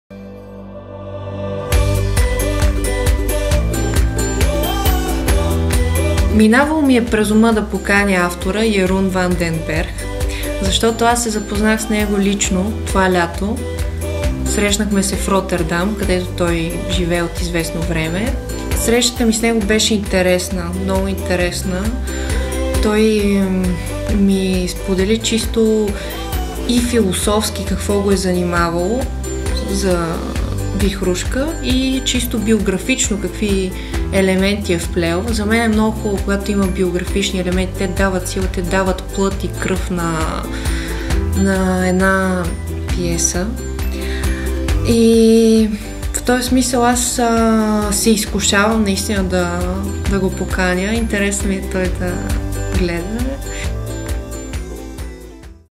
Александра Петрова - 7
Вихрушка
от Йерун ван ден Берх
Превод Нина Сиракова

Постановка Александра Петрова
Сценография Десислава Банкова
Участват Димитър Баненкин, Росица Александрова, Рада Кайрякова, Боян Арсов